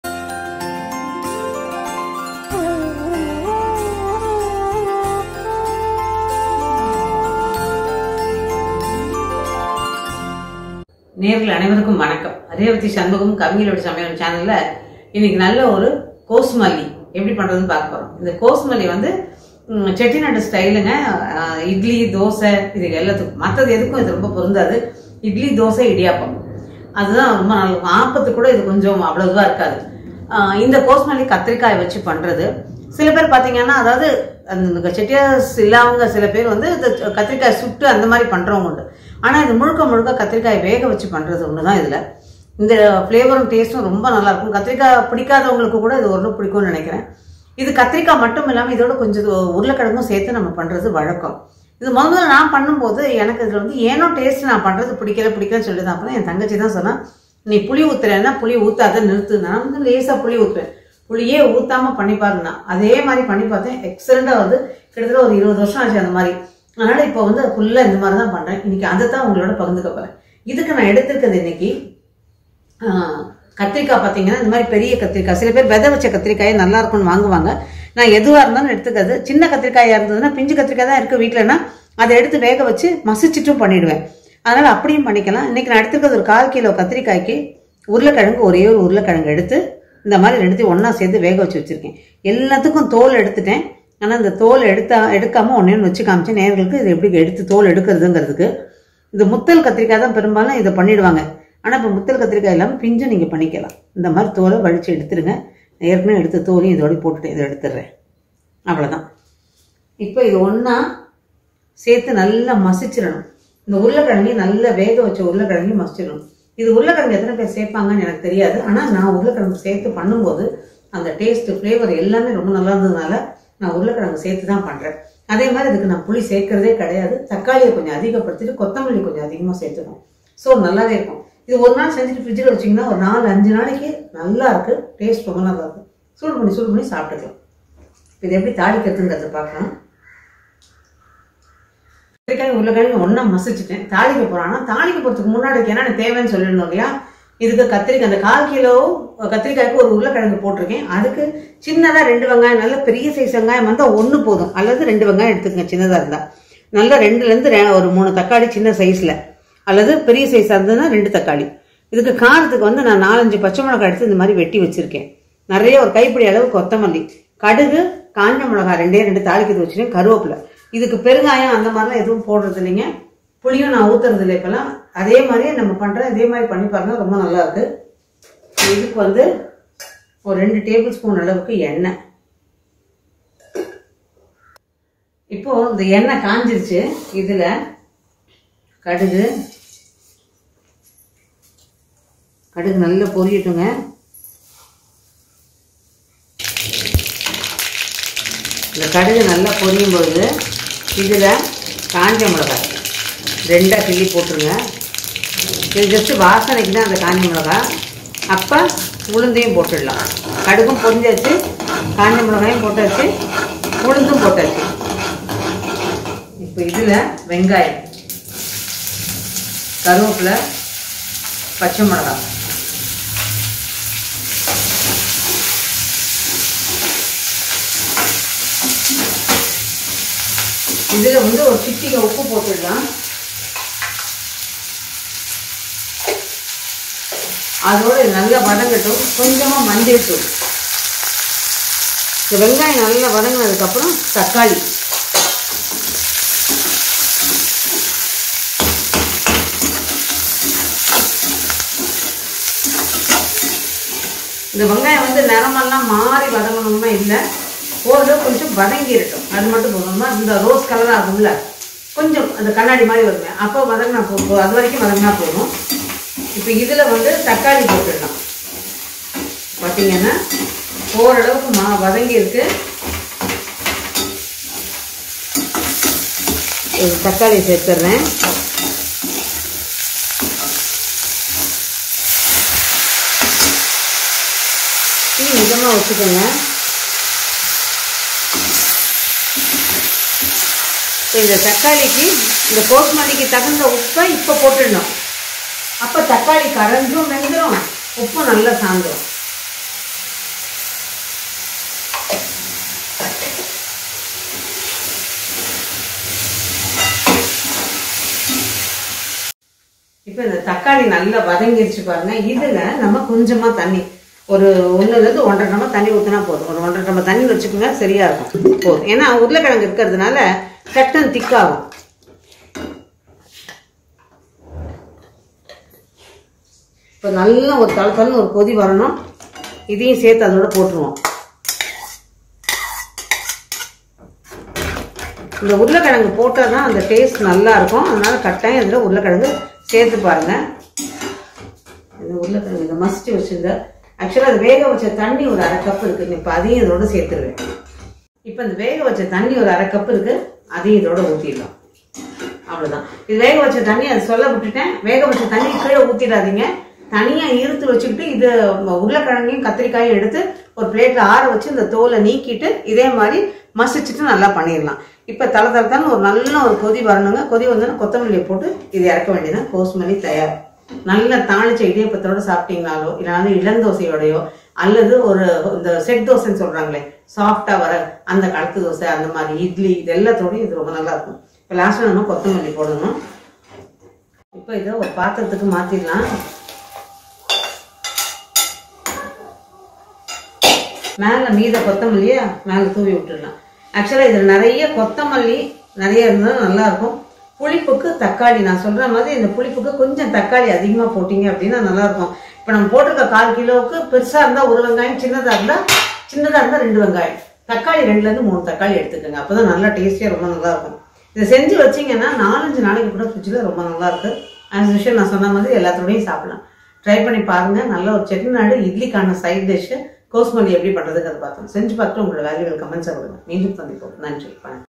Chettinad special - Kosumalli recipe by Revathy shanmugam
This video is about chettinad special - Kosumalli recipe

Brinjal -250 gms
Potato-1
Onion -1 (big)
Tomato-2 to 3
Urad dhal - 1 tsp
Coriander - some